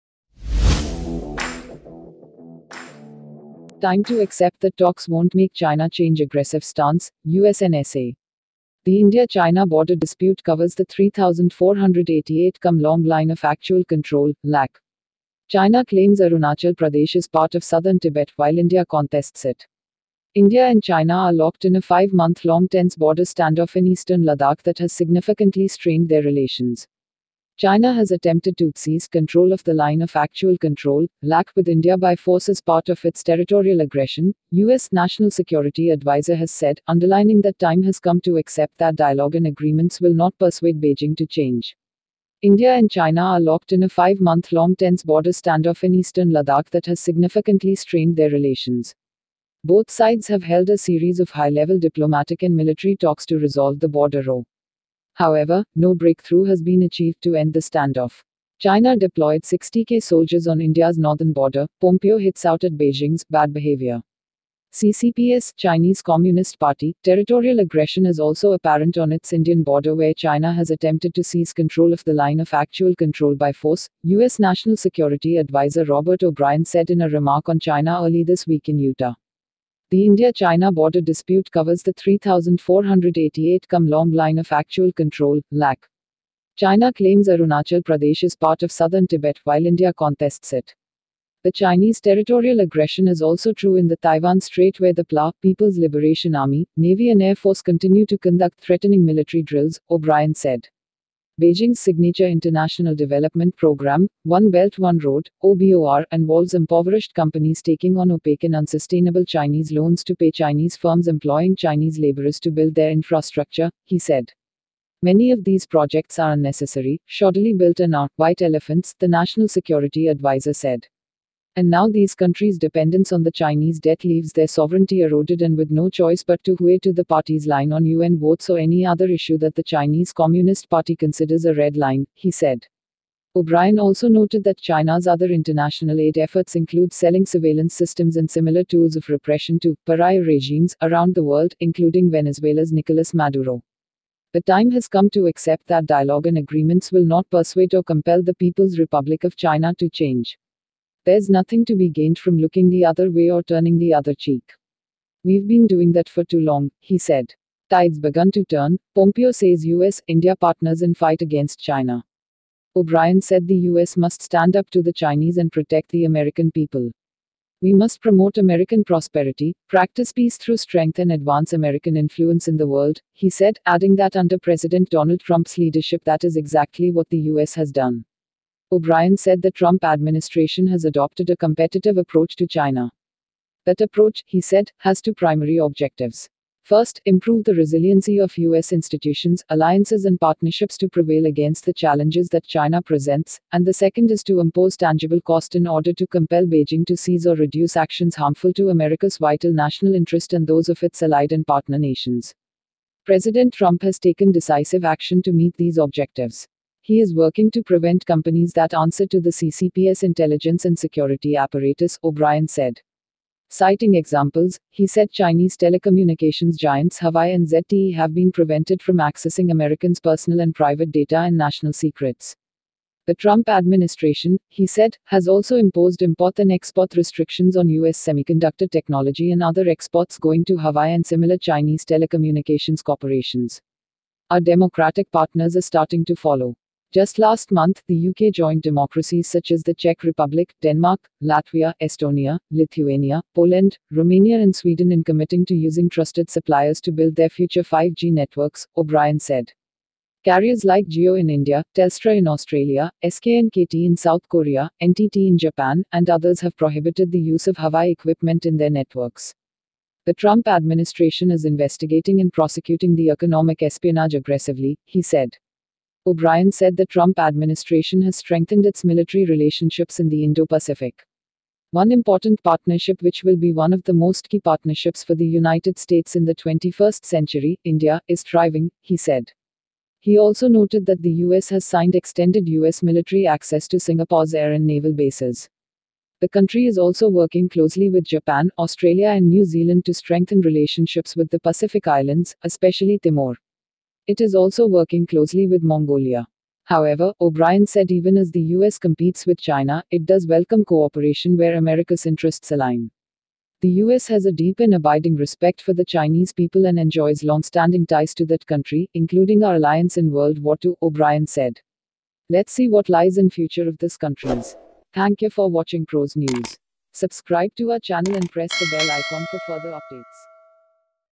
India China War Begins
Time to accept that talks won’t make China change aggressive stance: US NSA

The India-China border dispute covers the 3,488-km-long Line of Actual Control (LAC). China claims Arunachal Pradesh as part of southern Tibet, while India contests it.

Updated: Oct 10, 2020 15:07 IST

By Press Trust of India | Posted by Prashasti Singh, Washington

India and China are locked in a five-month-long tense border standoff in eastern Ladakh that has significantly strained their relations. (Reuters image)

China has attempted to “seize” control of the Line of Actual Control (LAC) with India by force as part of its territorial aggression, US’ national security advisor has said, underlining that time has come to accept that dialogue and agreements will not persuade Beijing to change.

India and China are locked in a five-month-long tense border standoff in eastern Ladakh that has significantly strained their relations. Both sides have held a series of high-level diplomatic and military talks to resolve the border row. However, no breakthrough has been achieved to end the standoff.

Also Read: ‘China deployed 60K soldiers on India’s northern border’: Pompeo hits out at Beijing’s ‘bad behaviour’

“CCP’s (Chinese Communist Party) territorial aggression is also apparent on its Indian border where China has attempted to seize control of the Line of Actual Control by force,” US National Security Advisor Robert O’Brien said in a remark on China early this week in Utah.

The India-China border dispute covers the 3,488-km-long Line of Actual Control (LAC). China claims Arunachal Pradesh as part of southern Tibet, while India contests it. The Chinese territorial aggression is also true in the Taiwan Strait where the PLA (People’s Liberation Army) Navy and Air Force continue to conduct threatening military drills, O’Brien said.

“Beijing’s signature international development programme, One Belt One Road (OBOR), involves impoverished companies taking on opaque and unsustainable Chinese loans to pay Chinese firms employing Chinese labourers to build their infrastructure,” he said.

Many of these projects are unnecessary, shoddily built and are “white elephants”, the national security advisor said. “And now these countries’ dependence on the Chinese debt leaves their sovereignty eroded and with no choice but to hue to the party’s line on UN votes or any other issue that the Chinese Communist Party considers a red line,” he said. O’Brien also noted that China’s other international aid efforts include selling surveillance systems and similar tools of repression to “pariah regimes” around the world, including Venezuela’s Nicolas Maduro. “The time has come to accept that dialogue and agreements will not persuade or compel the People’s Republic of China to change. There’s nothing to be gained from looking the other way or turning the other cheek. We’ve been doing that for too long,” he said.

Also Read: ‘Tide’s begun to turn’: Pompeo says US, India partners in fight against China

O’Brien said the US must stand up to the Chinese and protect the American people.

“We must promote American prosperity, practice peace through strength and advance American influence in the world,” he said, adding that under President Donald Trump’s leadership that is exactly what the US has done.

O’Brien said the Trump administration has adopted a competitive approach to China. That approach, he said, has two primary objectives. First, improve the resiliency of US institutions, alliances and partnerships to prevail against the challenges that China presents; and the second is to impose tangible cost in order to compel Beijing to cease or reduce actions harmful to America’s vital national interest and those of its allied and partner nations.

“President Trump has taken decisive action to meet these objectives. He is working to prevent companies that answer to the CCP’s intelligence and security apparatus,” O’Brien said.

Citing examples, he said Chinese telecommunications giants Huawei and ZTE have been prevented from accessing Americans’ personal and private data and national secrets. The Trump administration, he said, has also imposed import and export restrictions on US semiconductor technology and other exports going to Huawei and similar Chinese telecommunications corporations.

“Our democratic partners are starting to follow. Just last month, the UK joined democracies such as the Czech Republic, Denmark, Latvia, Estonia, Lithuania, Poland, Romania and Sweden in committing to using trusted suppliers to build their future 5G networks,” O’Brien said.

“Carriers like Jio in India, Telstra in Australia, SK and KT in South Korea, NTT in Japan, and others have prohibited the use of Huawei equipment in their networks. The Trump administration is investigating and prosecuting the economic espionage aggressively,” he said.

O’Brien said the Trump administration has strengthened its military relationships in the Indo Pacific.

“One important partnership